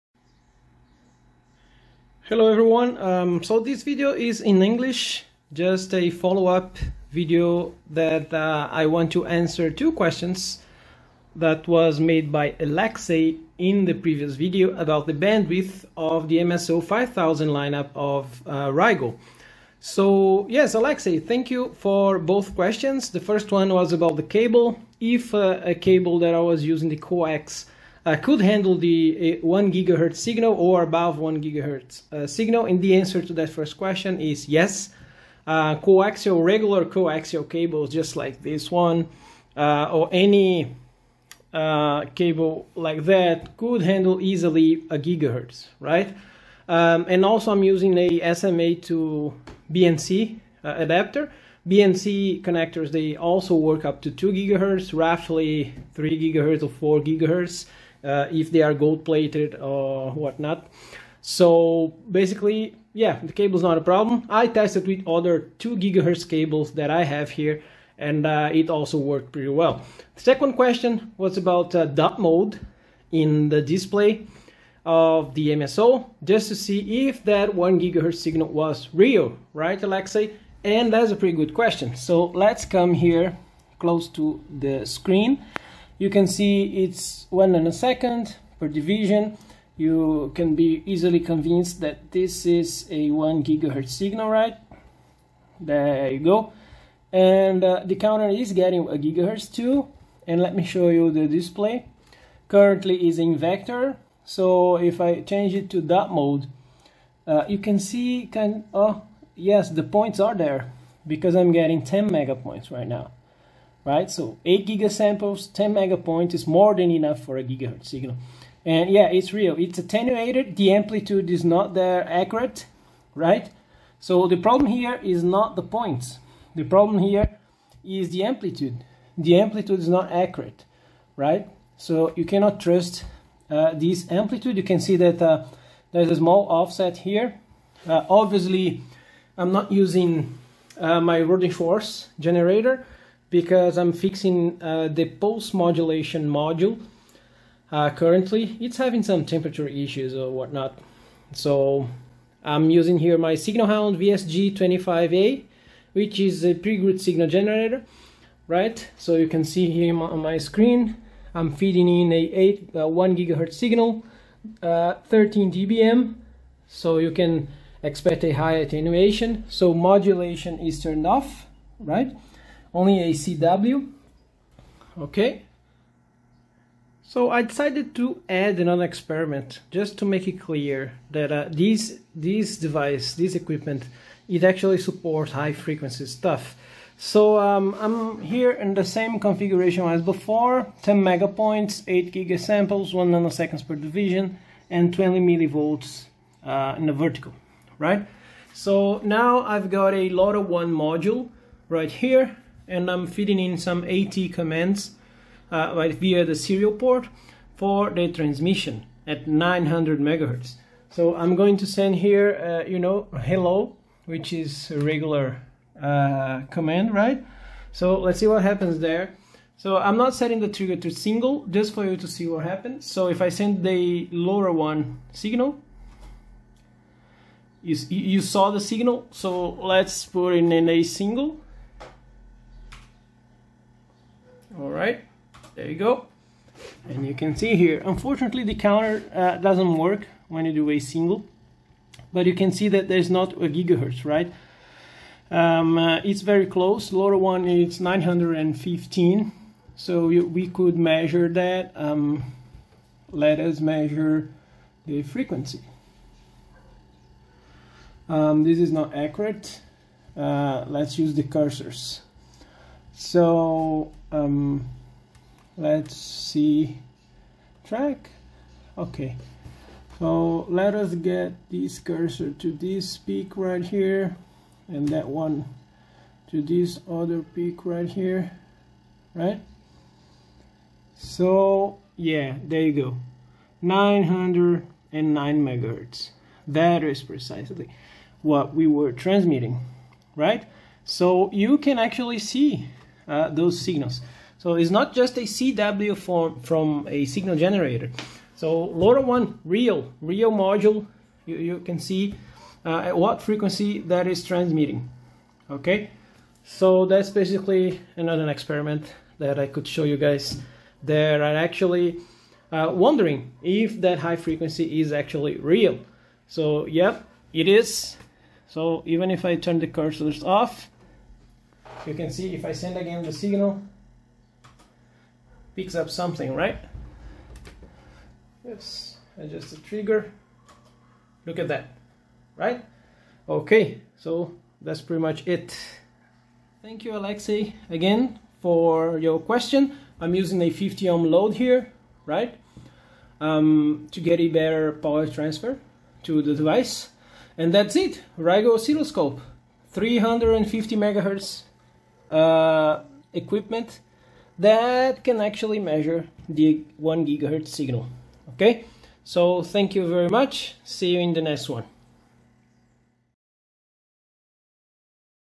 [EN] RIGOL MSO5000 BW Follow-up: 1GHz in dot mode
Can it handle 1 GHz in dot mode? How about real LoRaWAN signals?